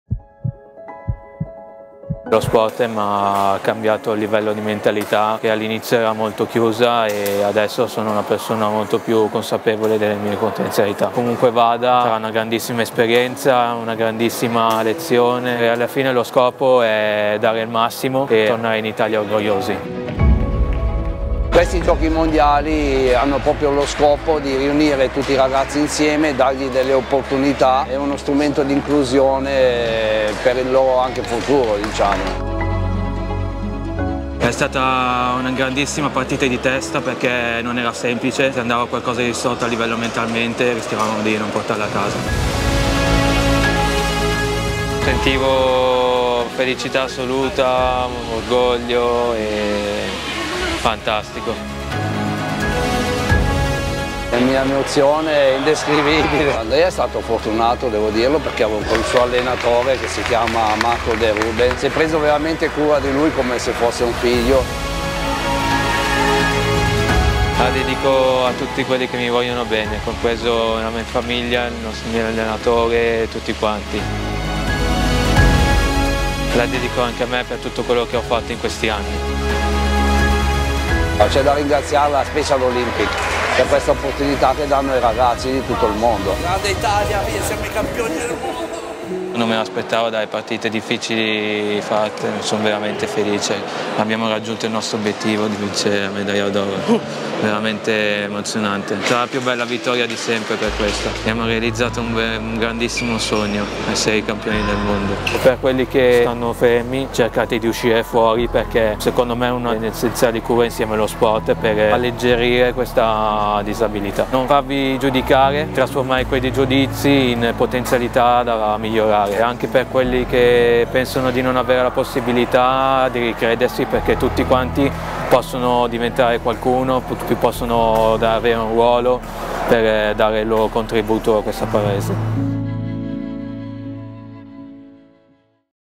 ORO BADMINTON ANDREA ALOISIO - Giochi Mondiali Special Olympics, Berlino 2023
Per quelle persone con disabilità e per le loro famiglie che stanno chiuse nel guscio familiare,  cercate di uscire, di fare sport. Tutti possono fare qualcosa, tutti possono dare il loro contributo nel mondo. È il messaggio di Andrea, Atleta azzurro del badminton che ha vinto la medaglia d'oro in coppia con Alessia Zucchelli. Bravo Andrea!!